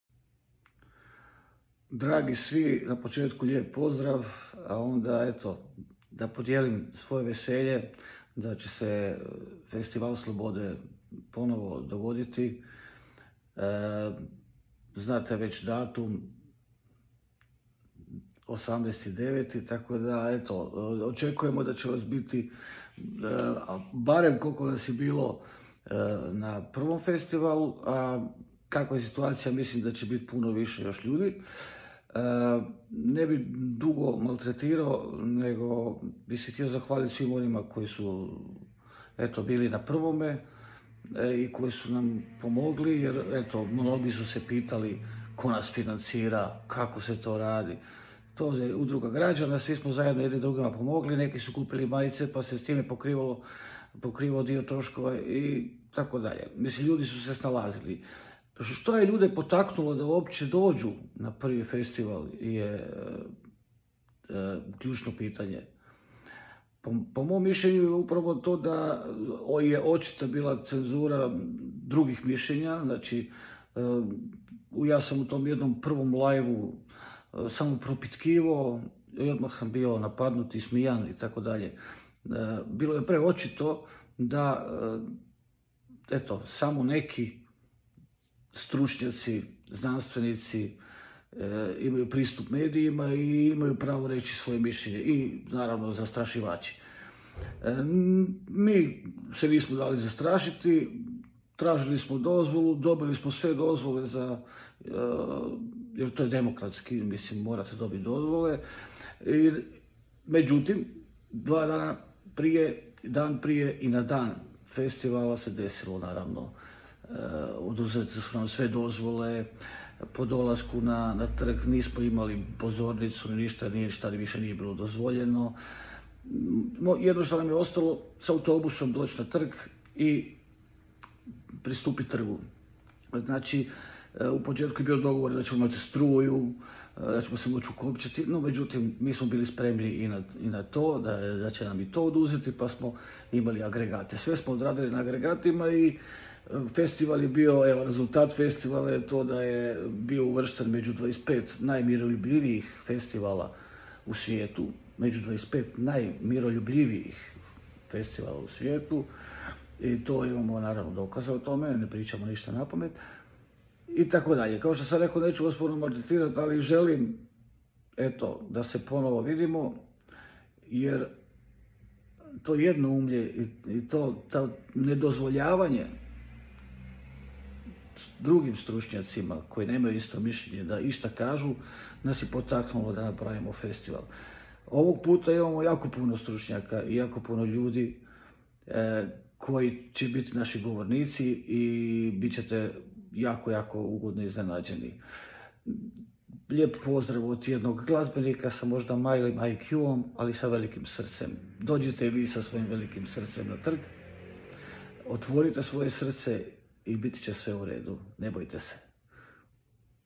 Festival slobode 2.0 - Tony Cetinski
Toni Cetinski poziva sve na Festival SLOBODE 2.0
18.rujna u Zagrebu!

Pratite nas na društvenim mrežama i ostalim kanalima te sami proslijedite informacije dalje.

Vidimo se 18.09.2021. na FestivalSlobode u Zagrebu